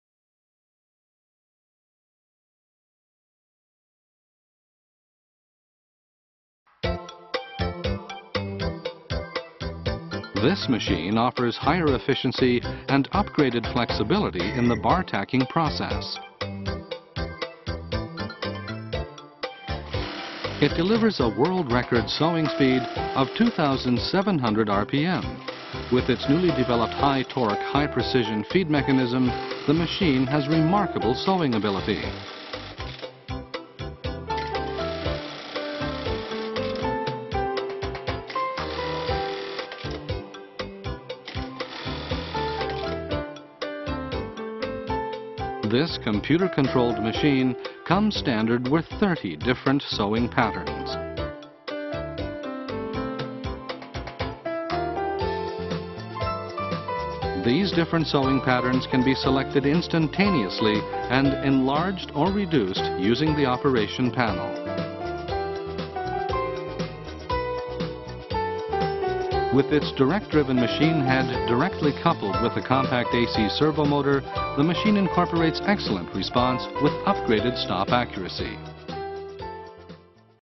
Juki LK-1900 закріплювальна машина з комп'ютерним керуванням
Juki LK-1900 високошвидкісна закріплювальна машина з комп'ютерним керуванням, що містить у пам'яті 30 різних шаблонів шиття, зручна панель керування для введення налаштувань, машинна головка з прямим приводом безпосередньо поєднана з компактним серводвигуном змінного струму.